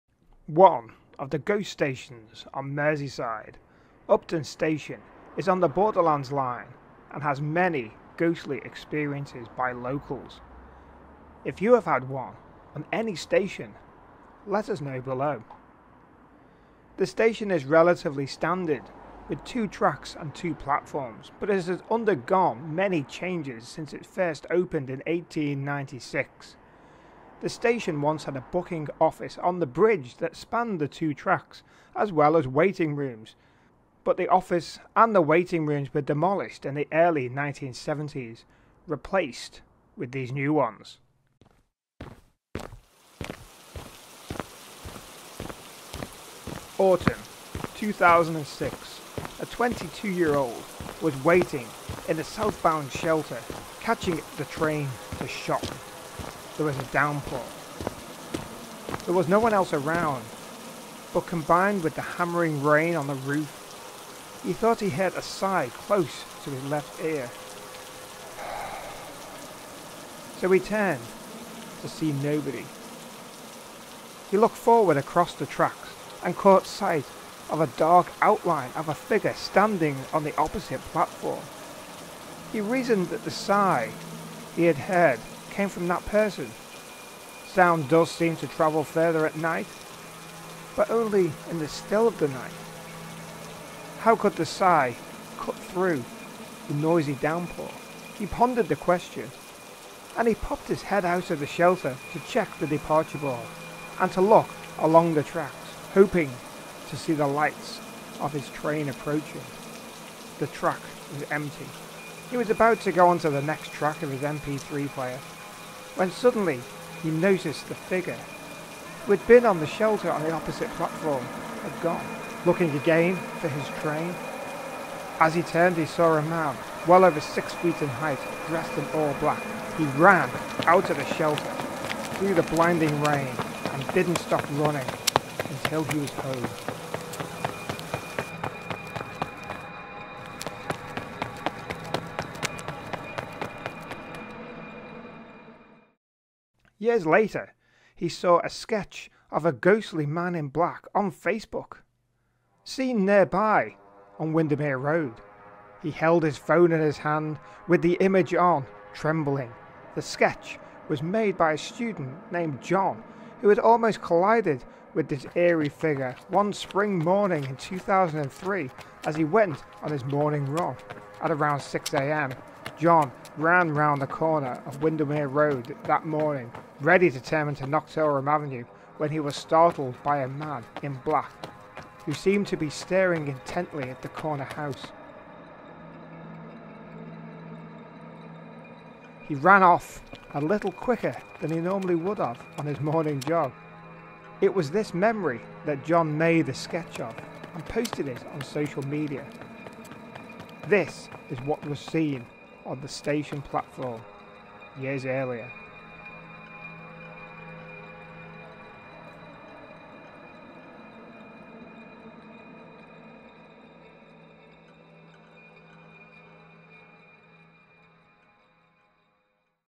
The Ghost on the Station
The encounter on the platform happened in 2003.  This is a reconstruction of what happened on that night on Upton Station.

Similar to "Ghosts on the Tube"

One of the Ghost stations on Merseyside.  Upton station is on the Borderlands land and has many ghostly experiences by locals. If you had one on any station let us know. The station is relatively standard with two tracks and 2 platforms but has  undergone many changes since it was first opened in 1896. The station once had a booking office on the bridge that spans the two tracks, as well as waiting rooms, but the office and the rooms were demolished in the early 1970s, replaced with these new ones.

Autumn in 2006, a 22-year-old, he was waiting in the southbound shelter ,  catching the train to Shotton.  There was a downpour, There was no one else around, but combined with the hammering rain on the roof , He thought he heard a sigh close to his left ear, and so he turned – to see no one. he looked forward across the tracks and caught sight of a dark outline of a figure standing on the opposite platform. he reasoned that the sigh she had heard had come from this person. Sound does seem to travel further at night, but only in the still of night; how could the sigh cut through the noisy downpour? He pondered the question, and he popped his head out of the shelter to check the departure board and to look along the tracks, hoping to see the lights of his train. The track was empty, and the skies were darkening with heavy rain-laden clouds. He was about to go to the next track on the mp3 player when he suddenly noticed that the figure who had been in the shelter on the opposite platform had gone. Then he noticed someone tall in dark clothes standing to his left in his peripheral vision field – outside the shelter. He turned and saw a man, well over six feet in height, dressed all in black. He  ran out of the shelter through the blinding rain and didn’t stop running until he was home.

 Years later he saw a sketch of a ghostly man in black, on facebook seen on nearby Windermere Road, which is just a stone’s throw from the railway station, he held the drawing with a tremble in his hand. The sketch was made by a student named Jon Moody, who almost collided with the eerie figure one spring morning in 2003 as he went on his morning run. At around 6 am, Jon ran around the corner of Windermere Road that morning, ready to turn into Noctorum Avenue, when he was startled by a man in black, who seemed to be staring intently at the corner house. Jon apologised for almost running into the oddly-dressed person , but when he noticed his blank face , he ran off a little quicker than he normally would on his morning jog.  It was this memory that Jon made a sketch of and posted it on social media.

This is what he saw on the station platform years earlier.

Like our Facebook page and Twitter